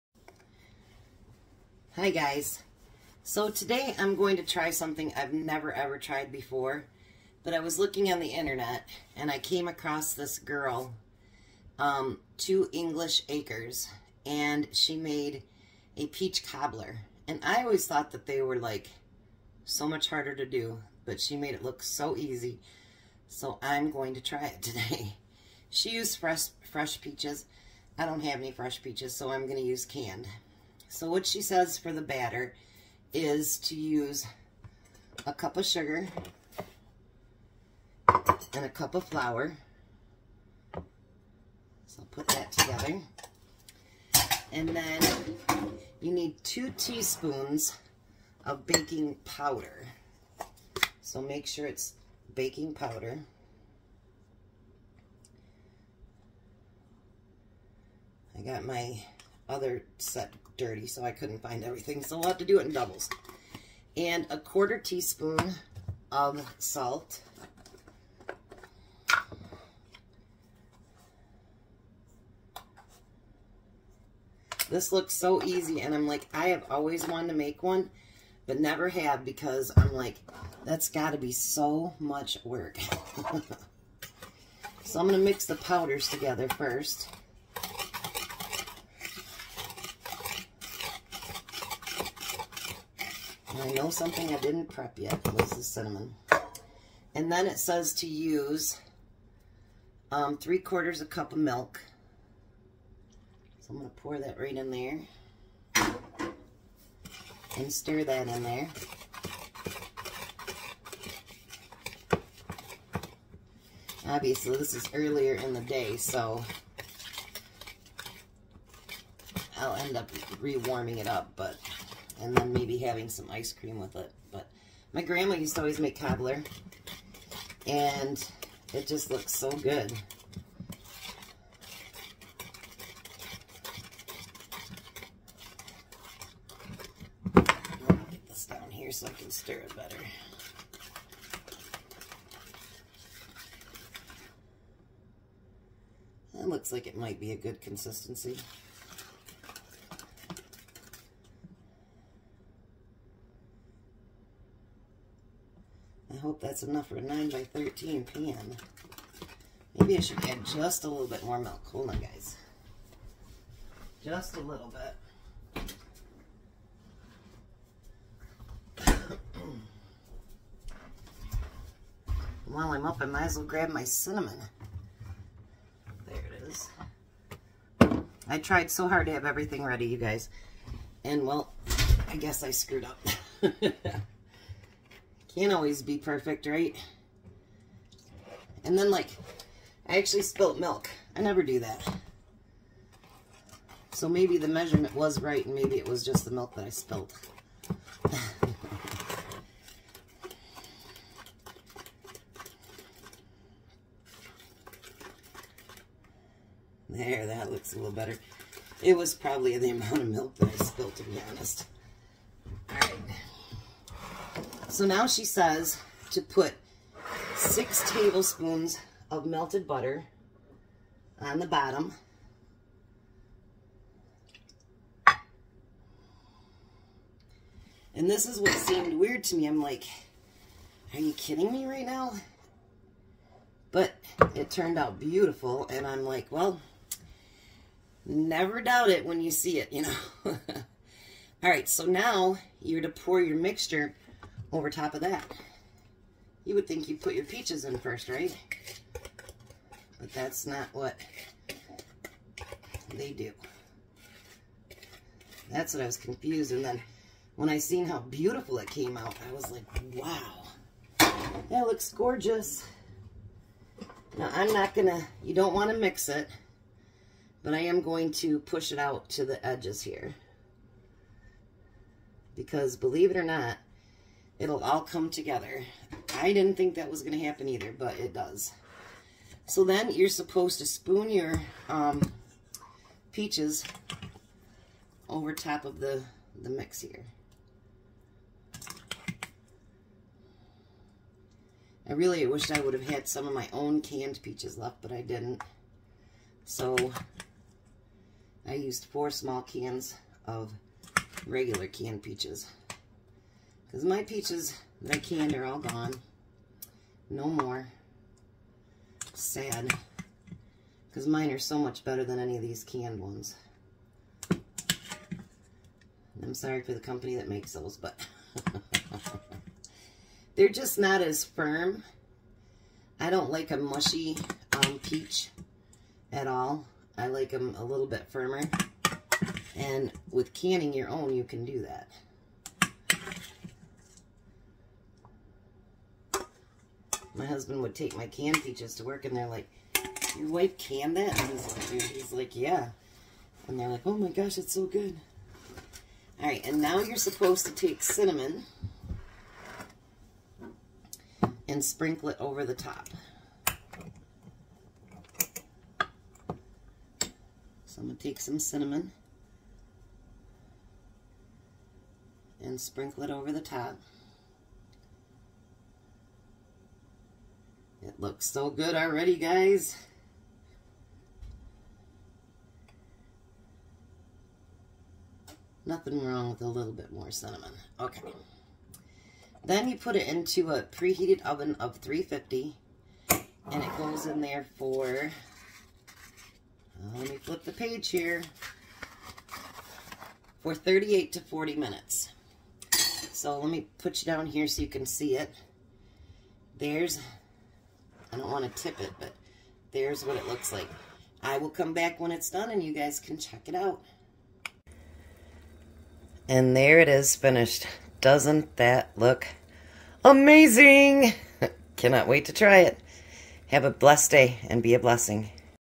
Peach cobbler thanks two English acres for the recipe
4 15 oz cans of peaches
3/4 cup granulated sugar
1/4 teaspoon salt
For the batter:
6 Tablespoons butter melted
1 cup all-purpose flour
1 cup granulated sugar
2 teaspoons baking powder
1/4 teaspoon salt
3/4 cup milk
ground cinnamon

 Preheat oven to 350 degrees F. Slice butter into pieces and add to a 9x13 inch baking dish. Place the pan in the oven while it preheats, to allow the butter to melt. Once melted, remove the pan from the oven.
 In a large bowl mix together the flour, sugar, baking powder, and salt. Stir in the milk, just until combined. Pour the mixture into the pan, over the melted butter and smooth it into an even layer.
 Spoon the peaches and juice (or canned peaches, if using) over the batter. Sprinkle cinnamon generously over the top.
 Bake at 350 degrees for about 45 minutes